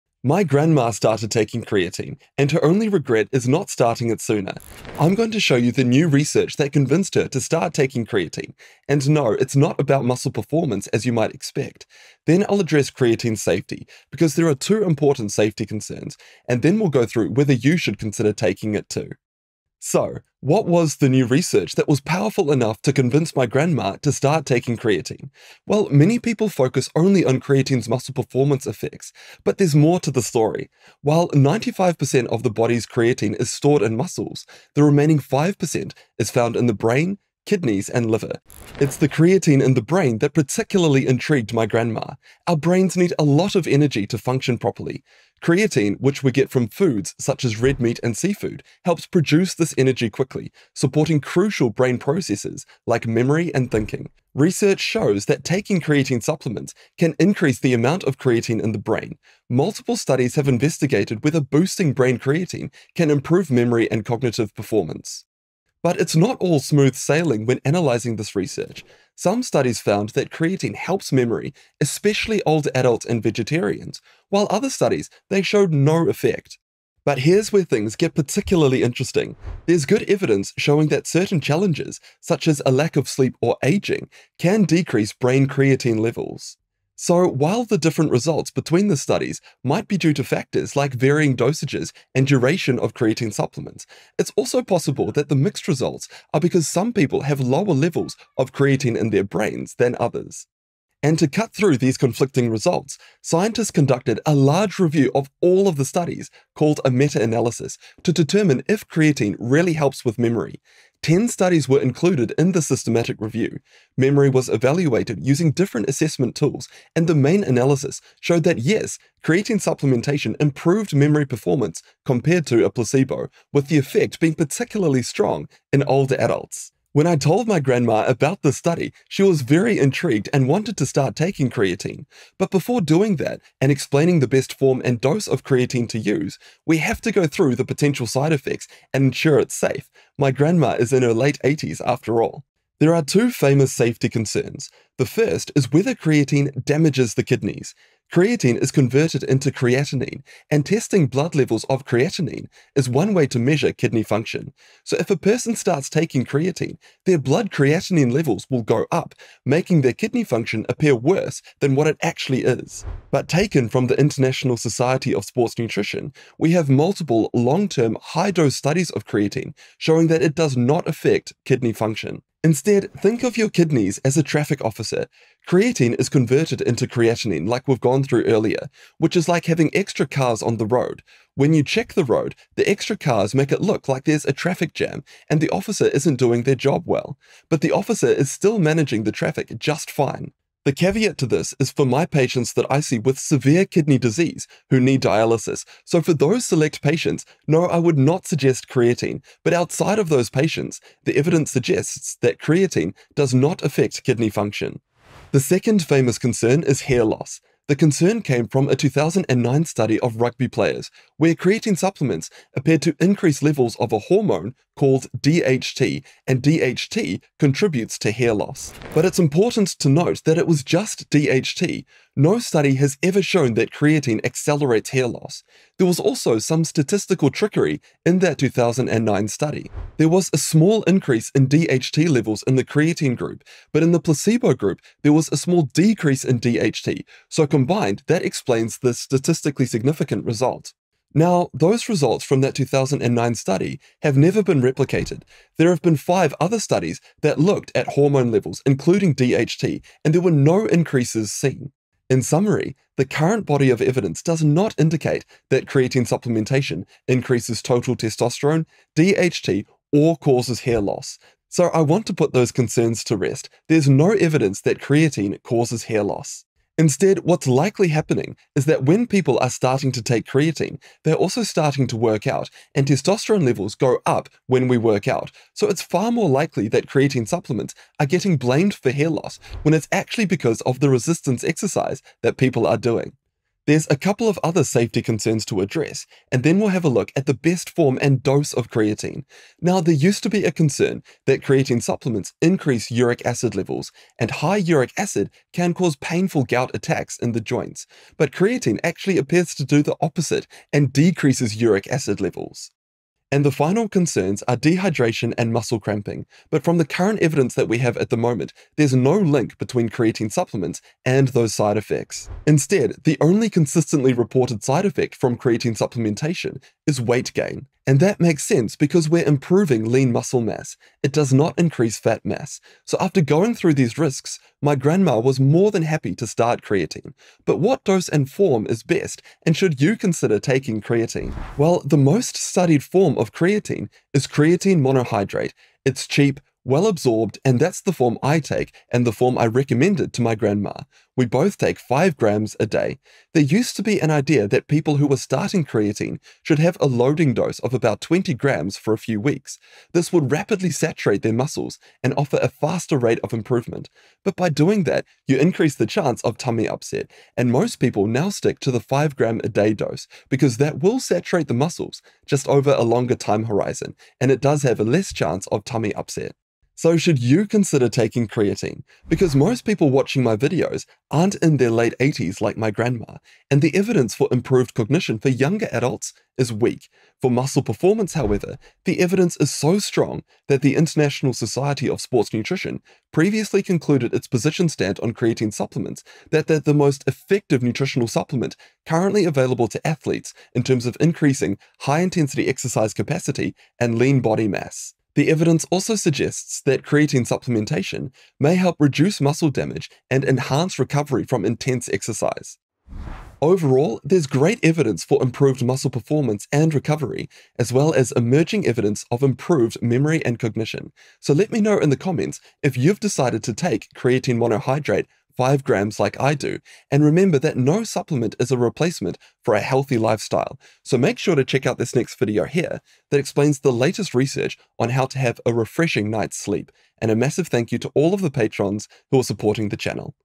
Why Even My Grandma Is Taking Creatine Daily!
My grandma started taking creatine, and her only regret is not starting sooner. This video explains why

For weekly health research summaries and extra sights, sign up